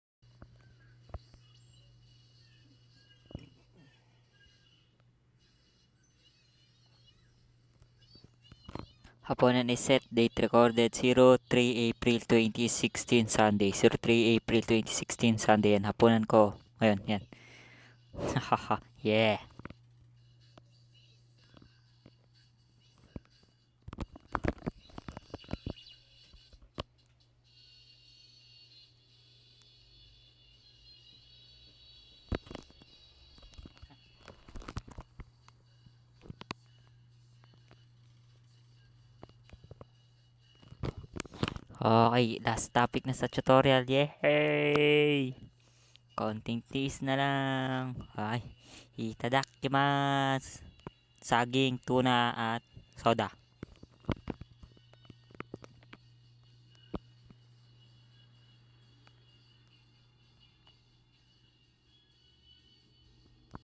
JD dinner set,tuna hot spicy and banana and Zesto Calamansi Can
Date Recorded : Sunday , 03 April 2016